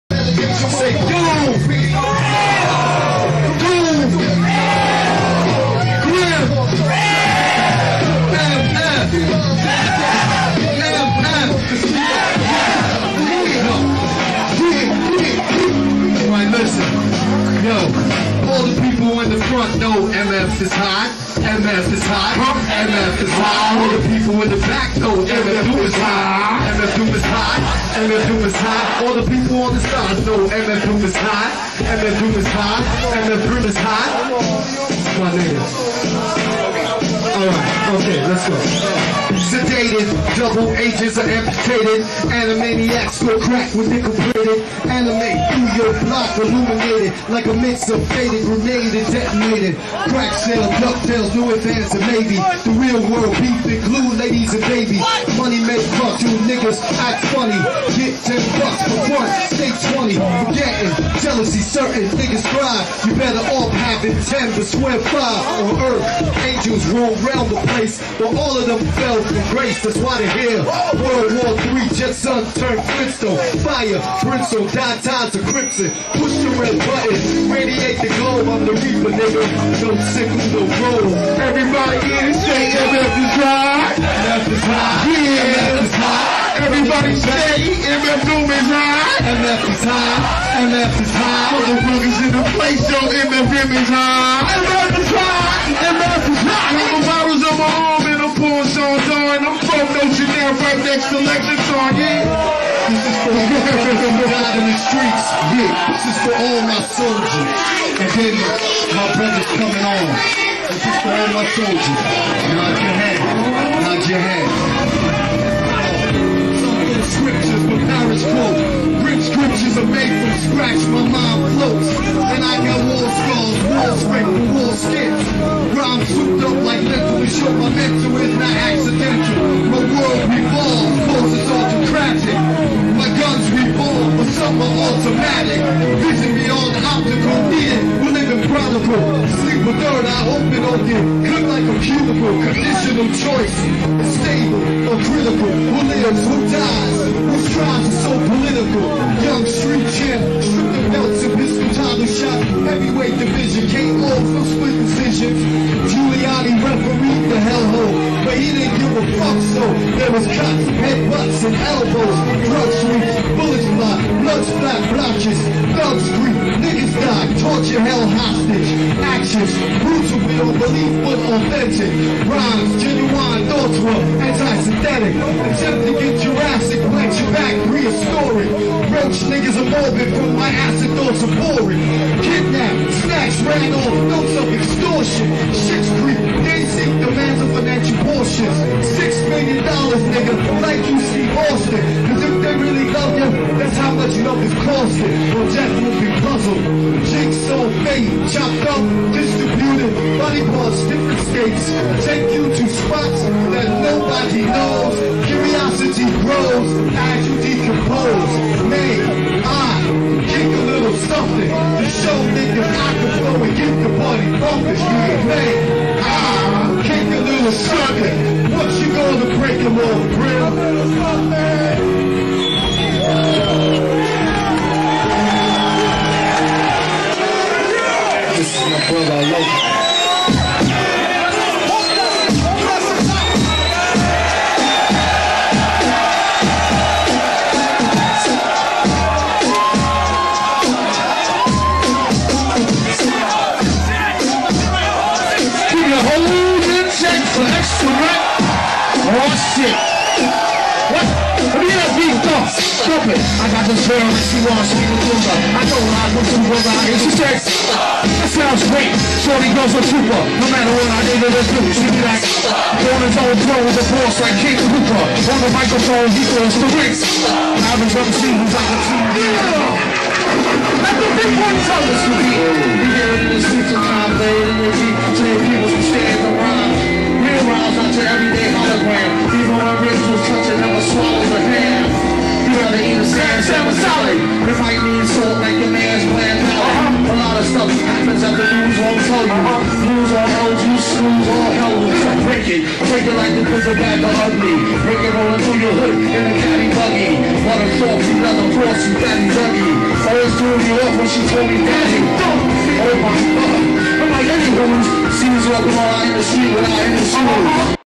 MF DOOM & MF GRIMM Live at BB Kings (1080p Upscale)
2021 remastered video & audio performance of MF DOOM & MF GRIMM   Live at BB Kings